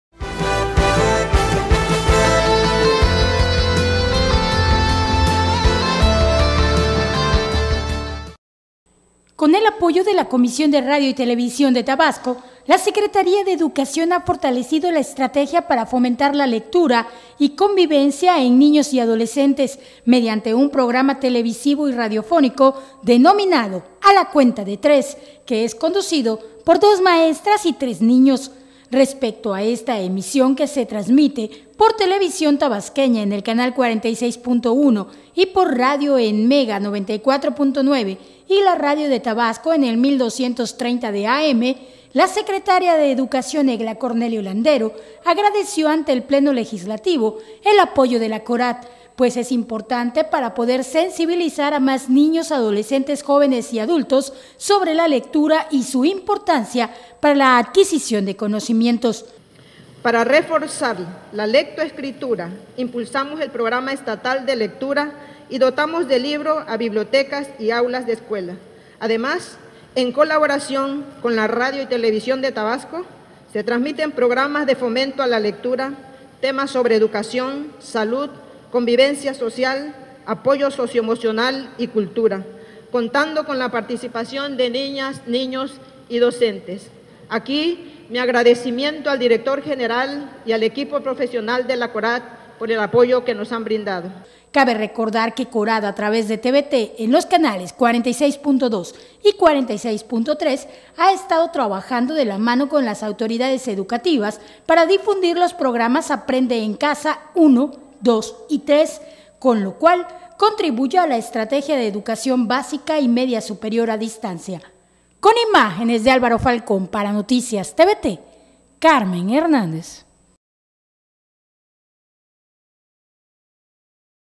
Reconoce Egla Cornelio apoyo de CORAT para fomentar la lectura en niños.
NoticiasTVT🟣 Fomentan la Secretaría de Educación y CORAT la lectura y convivencia en niños y adolescentes. La titular de @SETabasco , Egla Cornelio agradece ante el pleno del Congreso local, el apoyo de TVT y las radiodifusoras estatales.